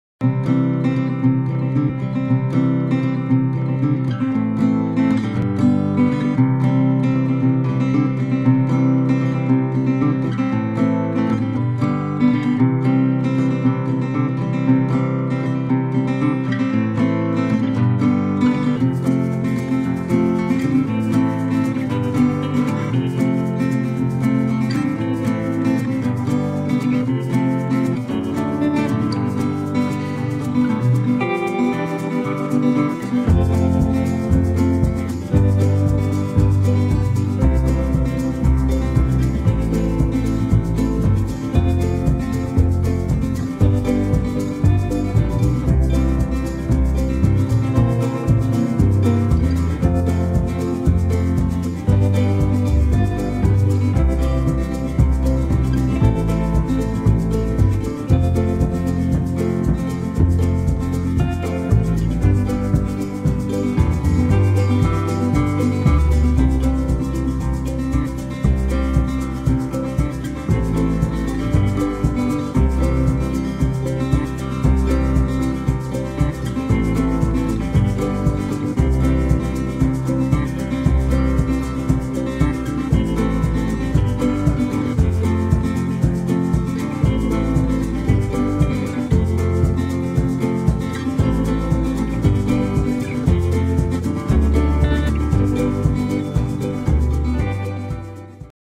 Kinzua Bridge 2017
Quick snippet of clear creek and farmers inn that burned down in the beginning, then went to visit Kinzua Bridge to top off the day.